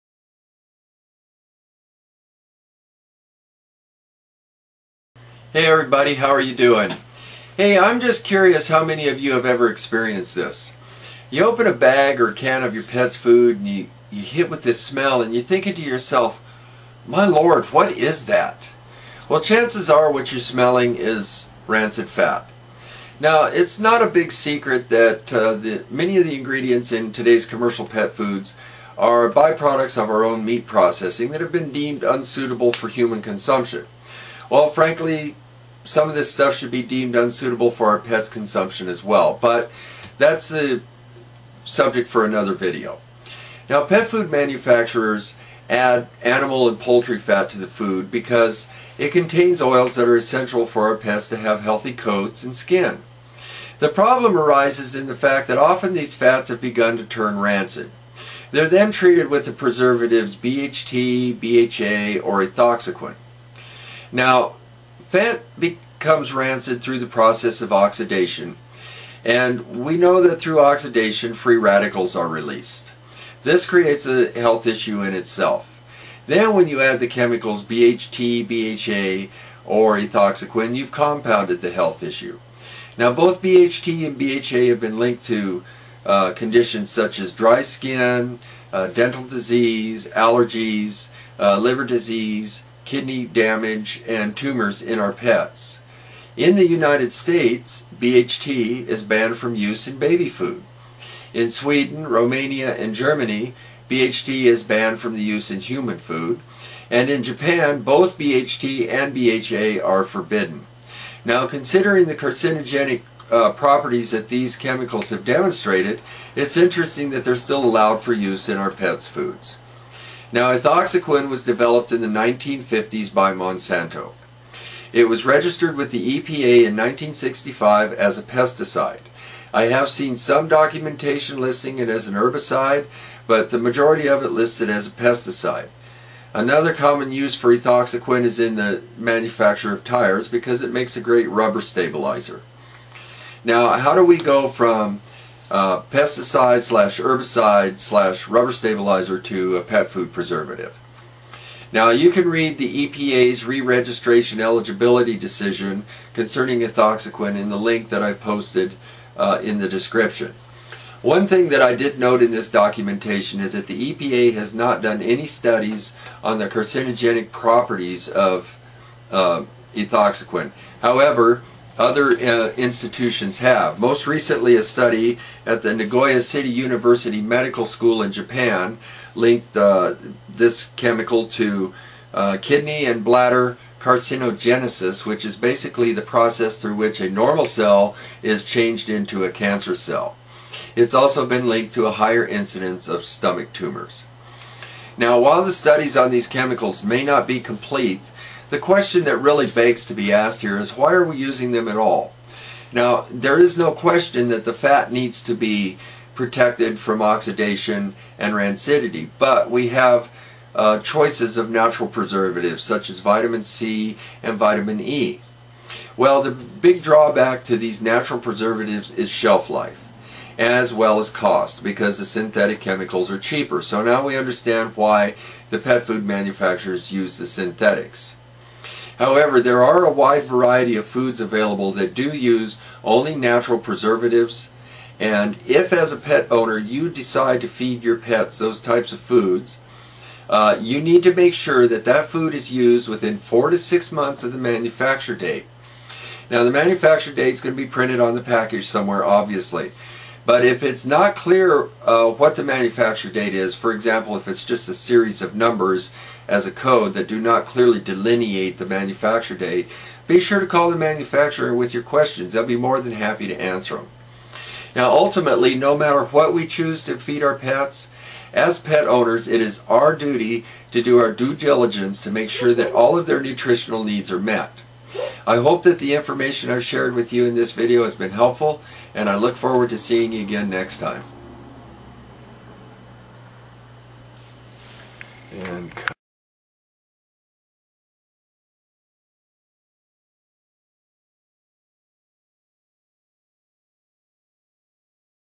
Perilous Additives In Pet Food
People love their pets and try to do the best for them. But what you don't know about their food can make them ill. Many of the preservatives used in todays pet foods have been linked to serious and even fatal illness in our pets. This is common knowledge with the pet food manufacturers yet the continue to use these chemicals. The worst of the three most commonly used preservatives is Ethoxyquin. Originally registered with the EPA in 1965 as a pesticide it is now a commonly used preservative in pet food. You can read the EPA's RED (Reregistration Eligibility Document) concerning Ethoxyquin at this